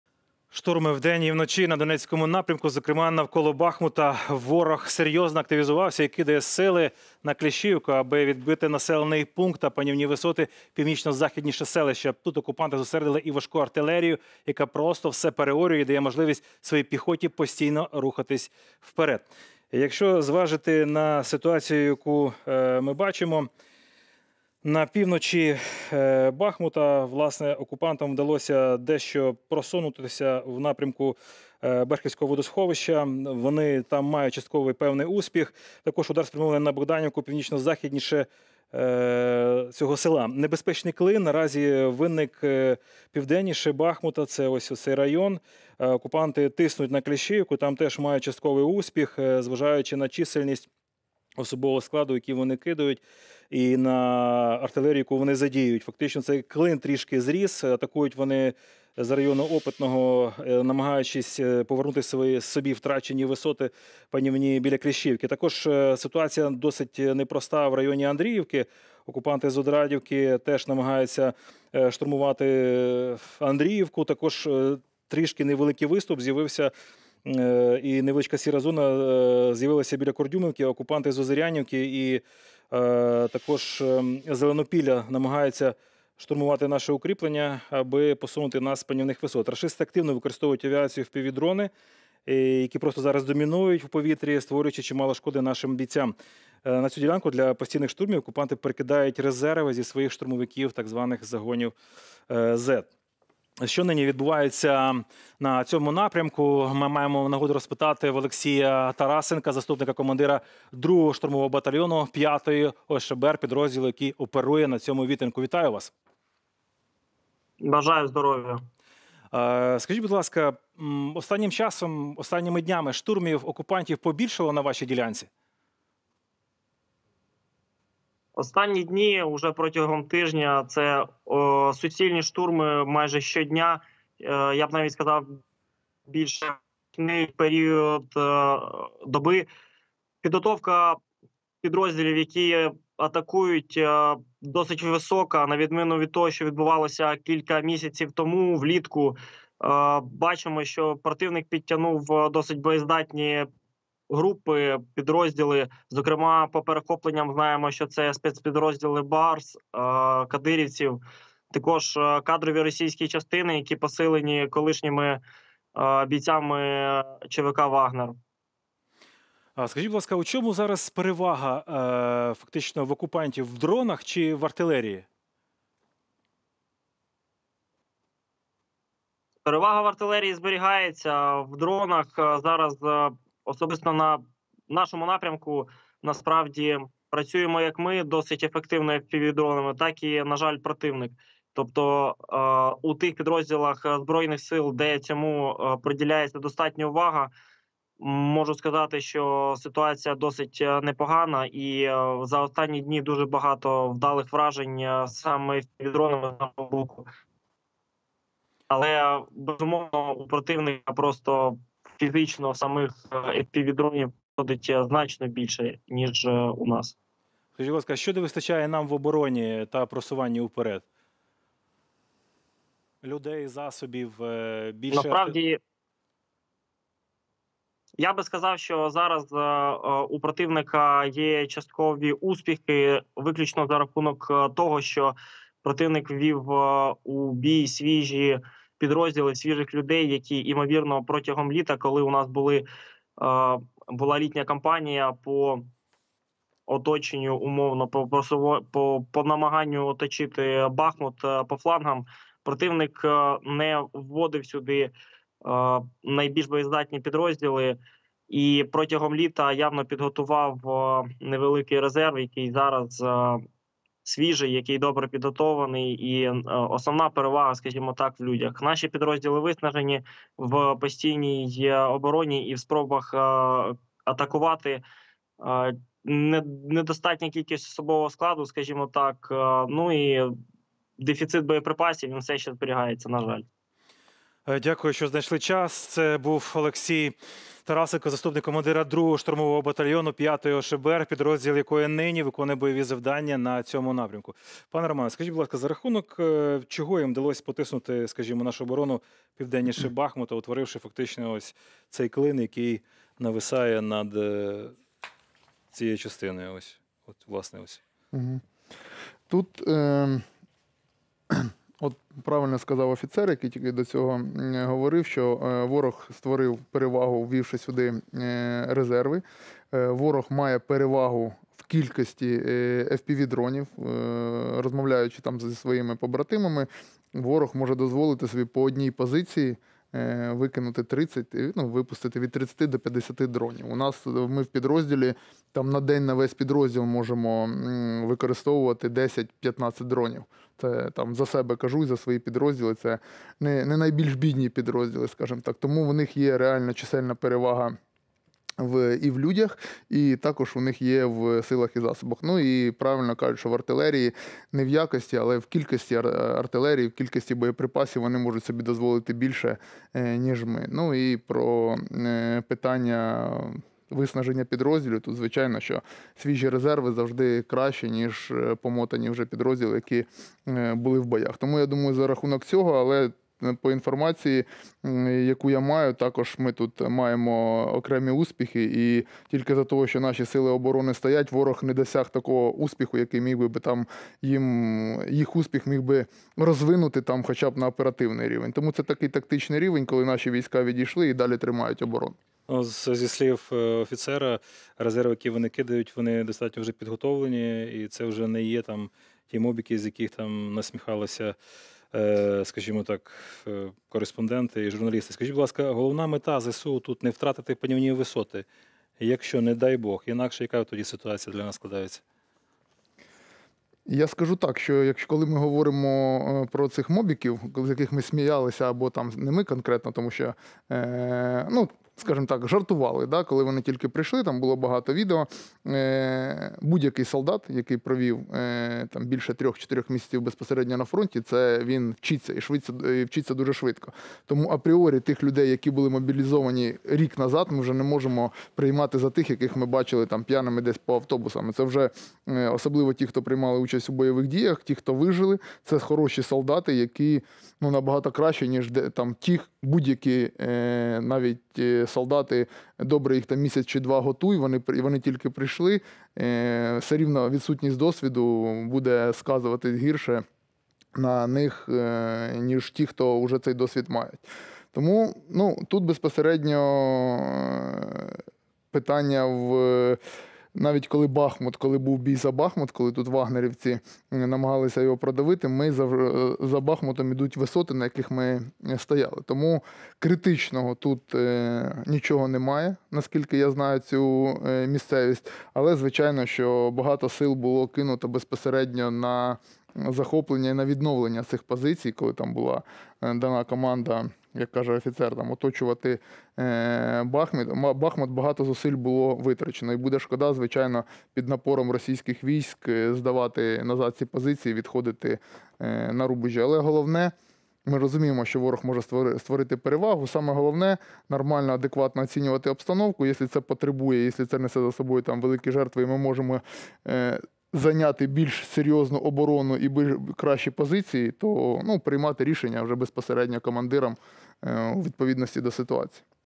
Бахмут, включення заступника командира 5-ої ОШБр Олексія Тарасенка,
Дивіться інформаційно-аналітичну програму Спецкор в оновленому форматі. Щотижня Спецкор: оперативний черговий на YouTube-каналі Спецкор.

Яка зараз оперативна ситуація навколо Бахмута? Про це говоритимемо з секретарем парламентського комітету з питань національної безпеки, оборони та розвідки - полковником Служби Безпеки України, Романом Костенком, наш військовий кореспондент, Руслан Ярмолюк, в оглядовому проєкті від редакції Спецкору: оперативний черговий.